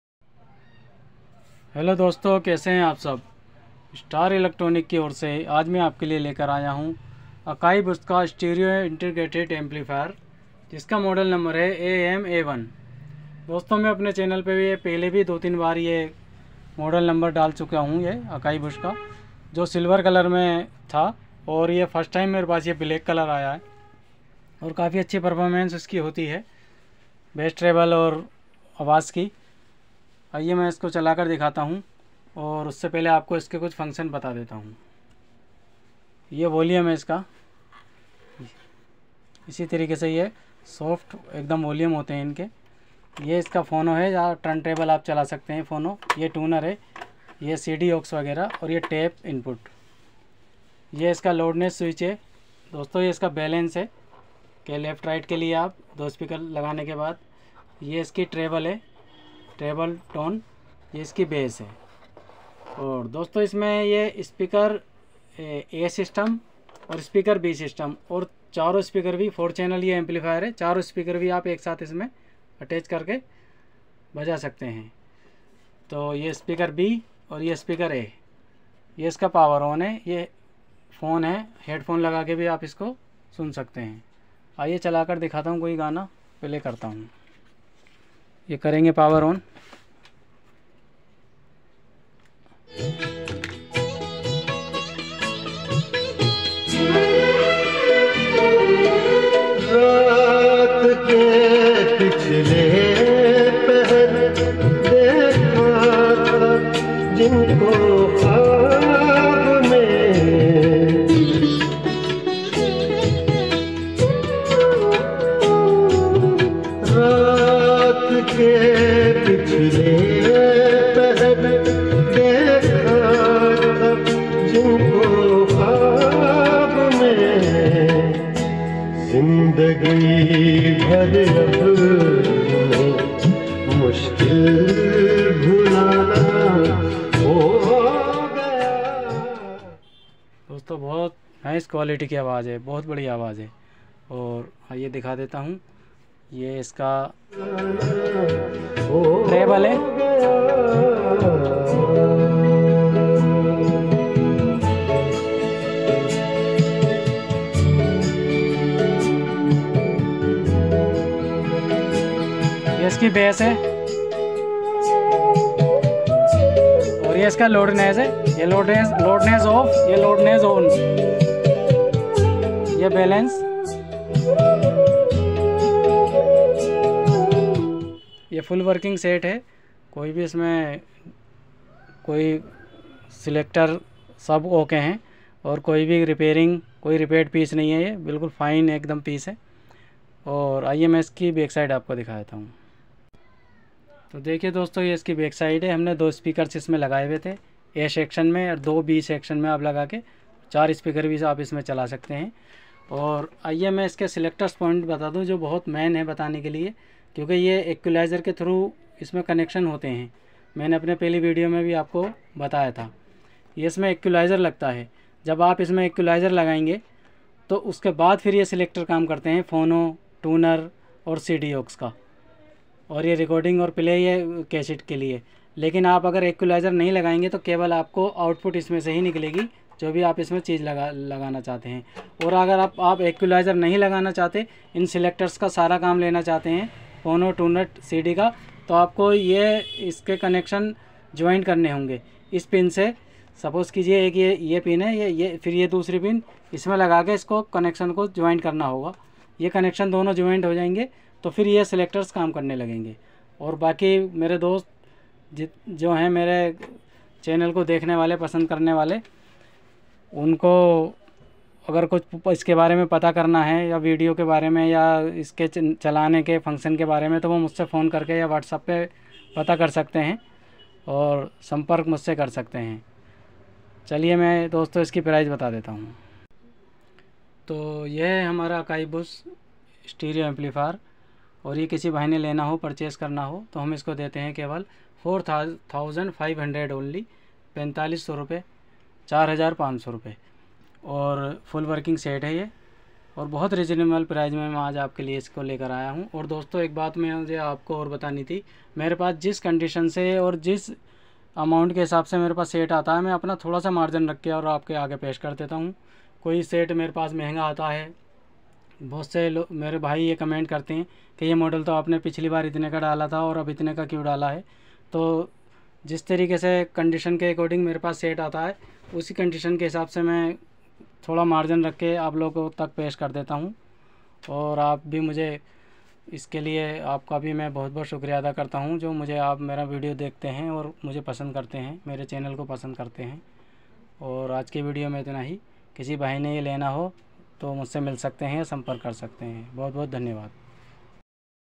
Sold. Price - 4500/- only AKAI BUSH STEREO INTEGRATED AMPLIFIER AM-A1
FOR SALE

AKAI BUSH STEREO INTEGRATED AMPLIFIER AM-A1

Akai
AKAI AMPLIFIER
Akai amplifier
Amplifier
amplifier
Stereo amplifier
Dolby
stereo
vintage
very nice condition
very nice sound Quality
AKAI